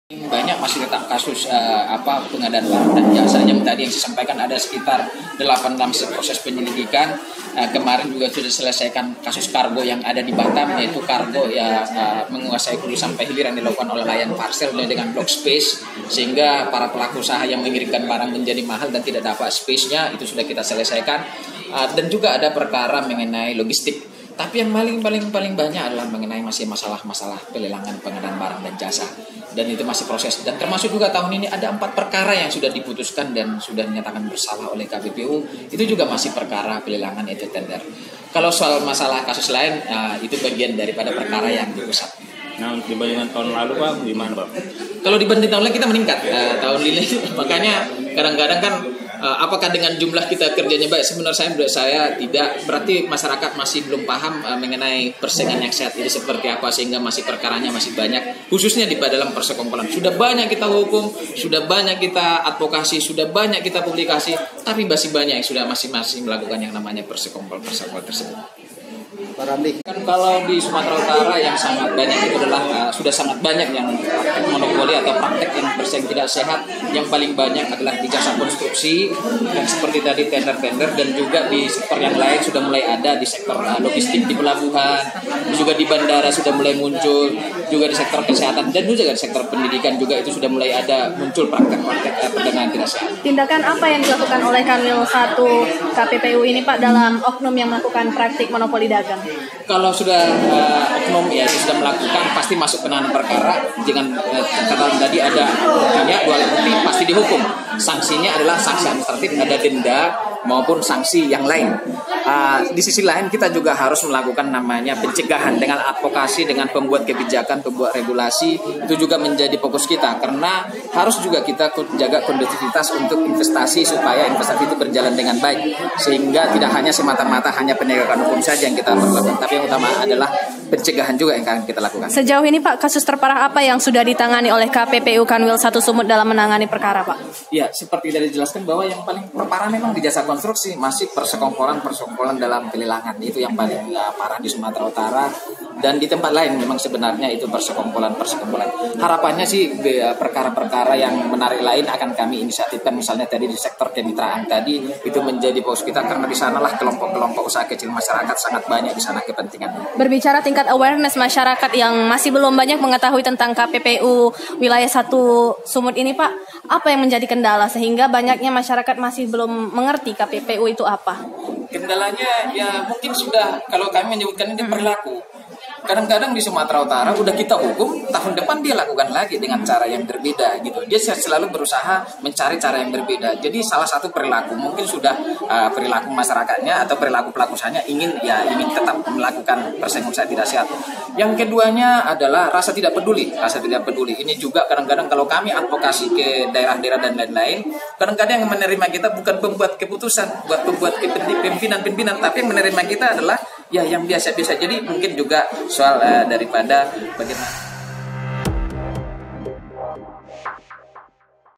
KPPU Ungkap Banyak Praktek Tidak Sehat Terkait Tender di Sumatera Utara
TRIBUN-MEDAN.COM, MEDAN- Selama tahun 2019, Komisi Pengawas Persaingan Usaha (KPPU) Kanwil I Medan melakukan delapan penyelidikan kasus, yakni tiga kasus non tender dan lima kasus tender.

Kepala Kanwil I KPPU, Ramli Simanjuntak mengatakan bila dibandingkan dengan tahun lalu, perkara tahun 2019 ini meningkat, apalagi berkaitan dengan persekongkolan tender.

"Di Sumatera Utara banyak praktek yang tidak sehat paling banyak jasa konstruksi dan tender-tender," kata Ramli di Kantor Kanwil I KPPU Medan, Jumat (27/12/2019).

 "Sudah banyak kita hukum. Sudah banyak kita advokasi dan publikasi, tapi masih banyak yang melakukan persekongkolan tersebut," sambungnya.

Diakuinya, sektor logistik di pelabuhan, bandara, dan sektor kesehatan bahkan pendidikan sudah muncul praktek-praktek perdagangan tidak sehat.

Ramli menjelaskan adapun delapan penyelidikan kasus yakni mengenai jasa pengiriman logistik dan pos di Batam (Kepri), preservasi rutin Jalan Kambang Tapan (Sumbar), peningkatan jaringan irigasi di Bandar Sidoras (Sumut).